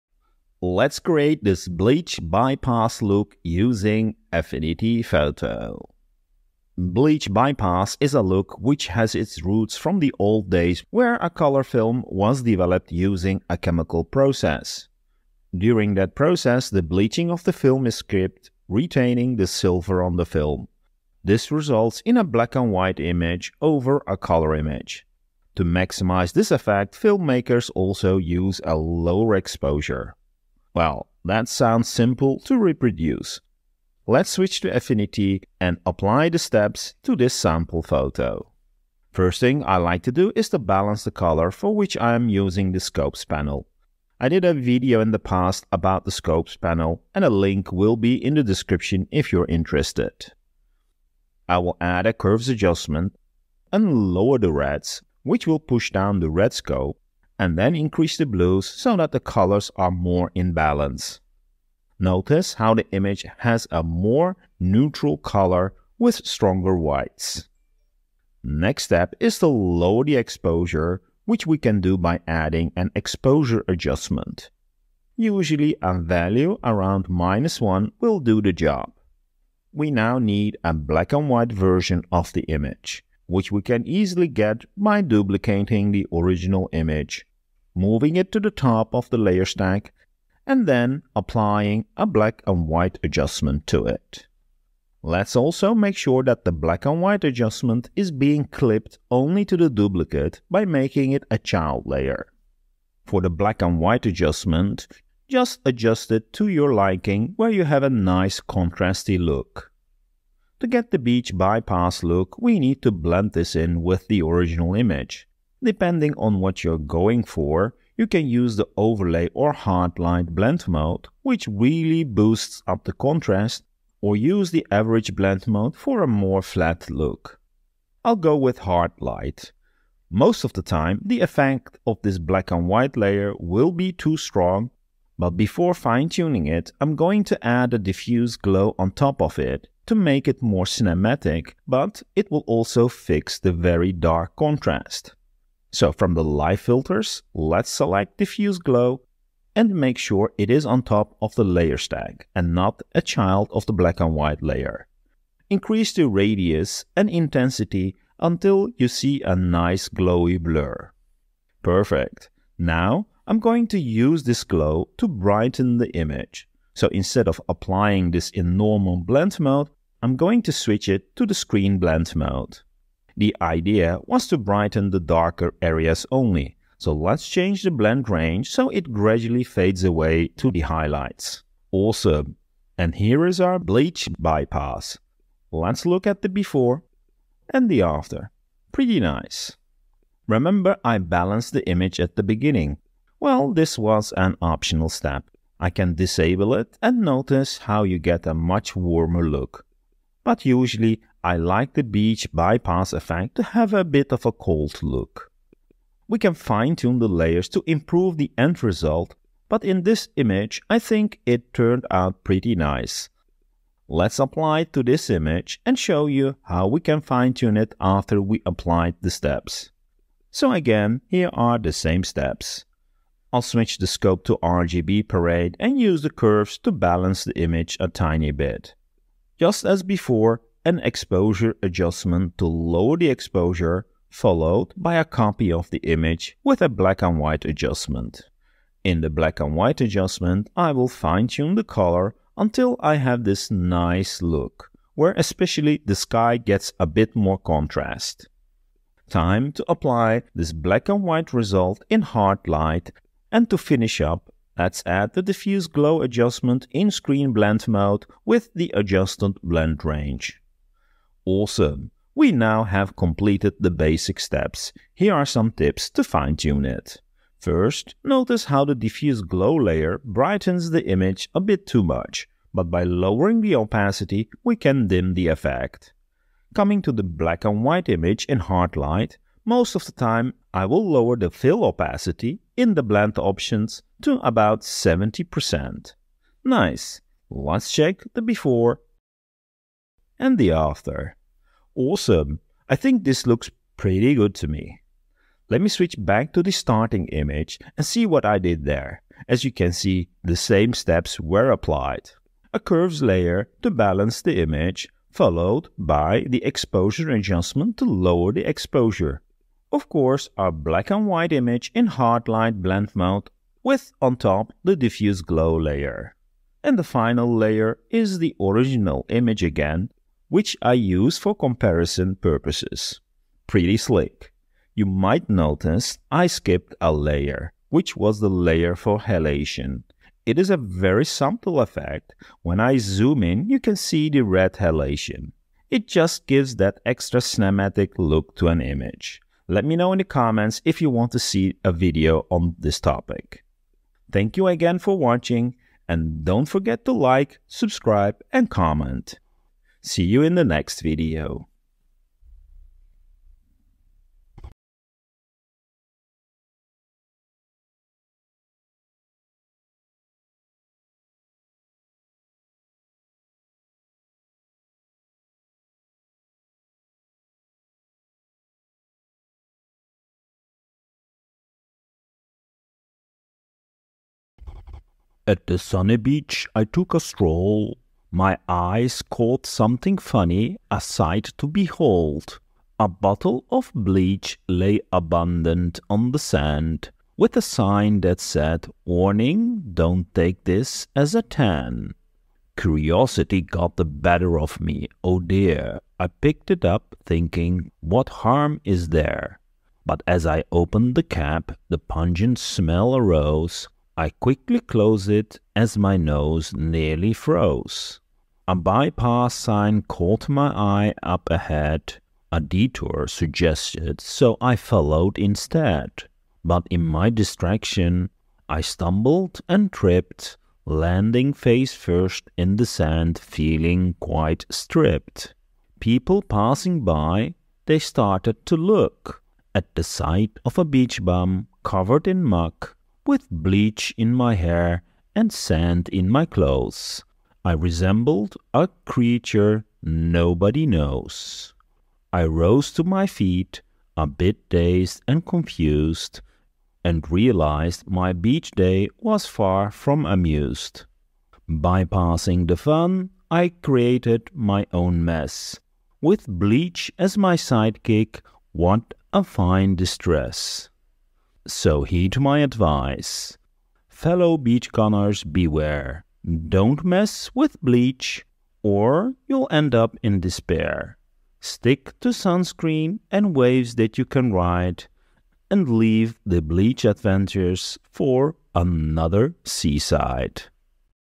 Cinematic Bleach Bypass like SPIELBERG look using Affinity Photo
The bleach bypass is a look which can be applied fairly easy to your photos to give it an artistic look. It is a  look that has been used multiple times by Steven Spielberg (Minority Report, Saving Private Ryan) and many more other filmmakers.